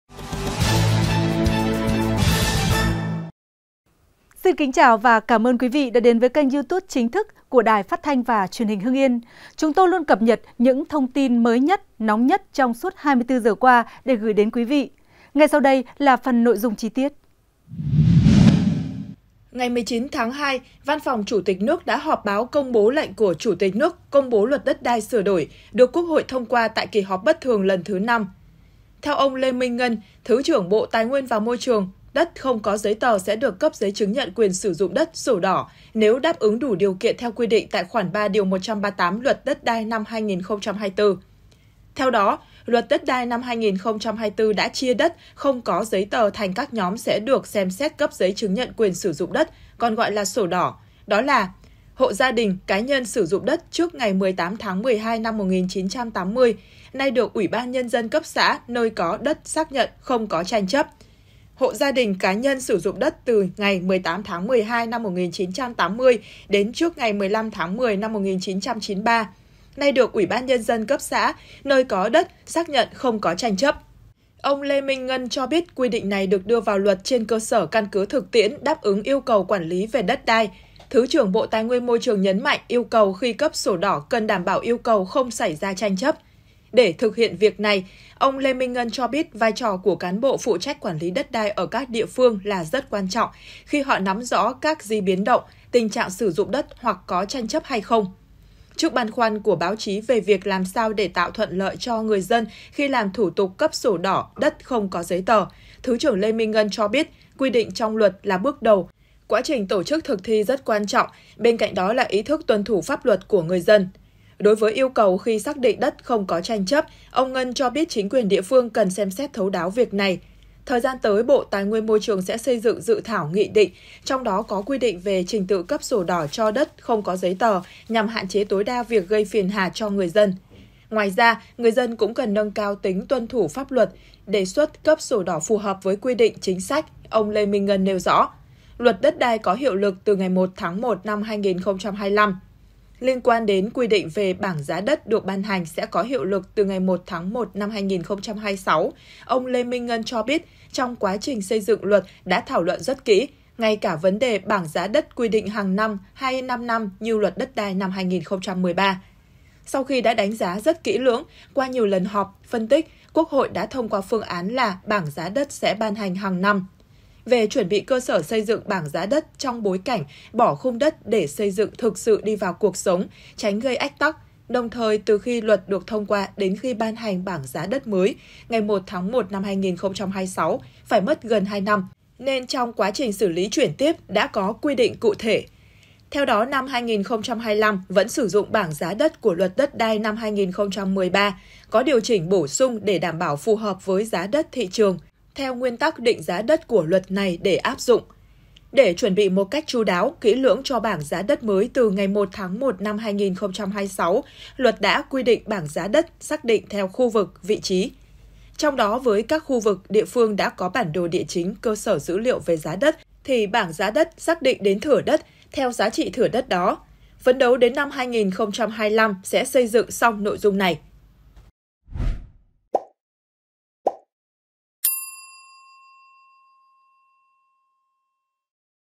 Cấp sổ đỏ cho đất không giấy tờ: Lãnh đạo Bộ TN-MT thông tin mới nhất về Luật Đất đai sửa đổi
Hưng Yên TV | Cấp sổ đỏ cho đất không giấy tờ: Lãnh đạo Bộ TN-MT thông tin mới nhất về Luật Đất đai sửa đổi
Ngày 19-2, Văn phòng Chủ tịch nước đã họp báo công bố lệnh của Chủ tịch nước công bố Luật Đất đai (sửa đổi), được Quốc hội thông qua tại kỳ họp bất thường lần thứ 5.
Địa chỉ: 164 Nguyễn Văn Linh, thành phố Hưng Yên, tỉnh Hưng Yên
Tiktok: Truyền hình Hưng Yên
Facebook: Đài Phát thanh và Truyền hình Hưng Yên
Zalo: Đài PTTH Hưng Yên